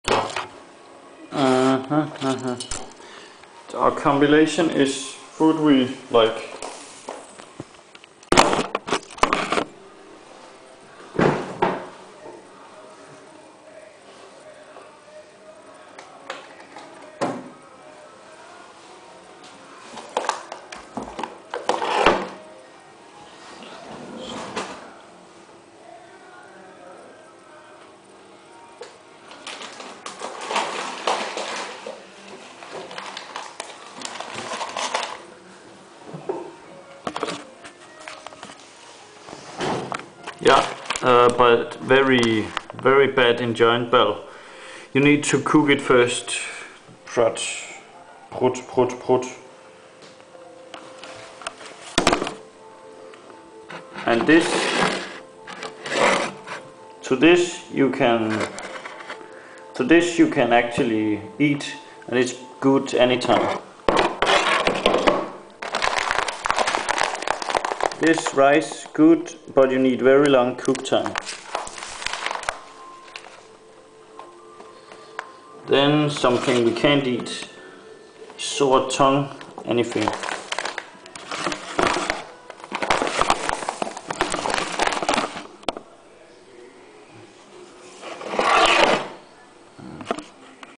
Enjoin able food except pi ananas.
Det viser hvilken mad her der kan nydes fuldt ud.
Det var mest kompliceret at skulle koge op spaghetti .